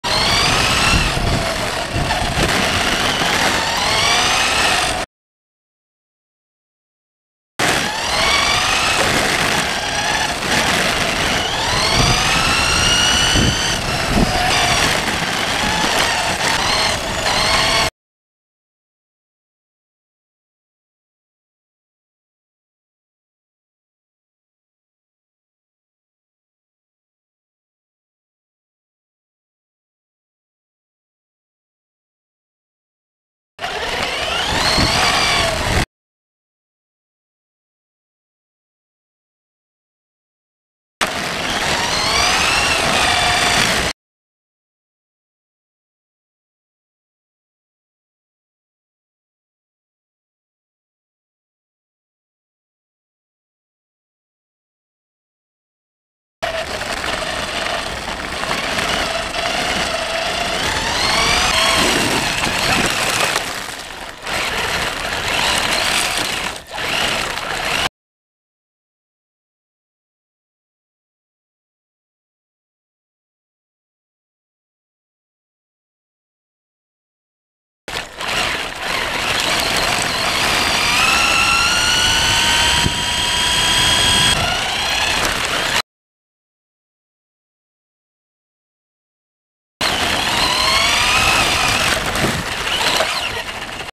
RC Racing - RC10
RC-körning på gatan, med GoPro